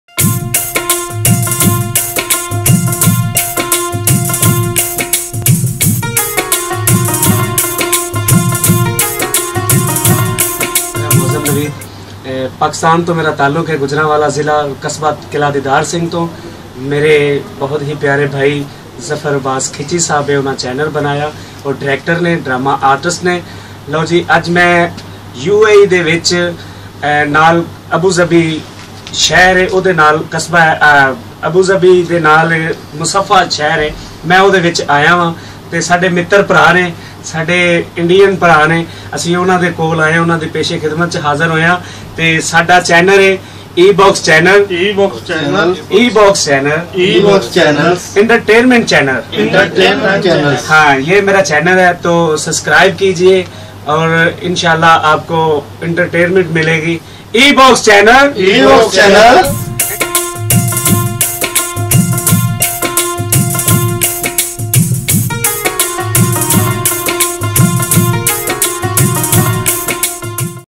معظم علی معروف ٹی وی سٹیج کامیڈین کا UAEسے ظفر عباس کھچی کو E-Box TVکی لانچنگ پر مبارک باد
Mouazam Ali Famous Tv Stage Comedian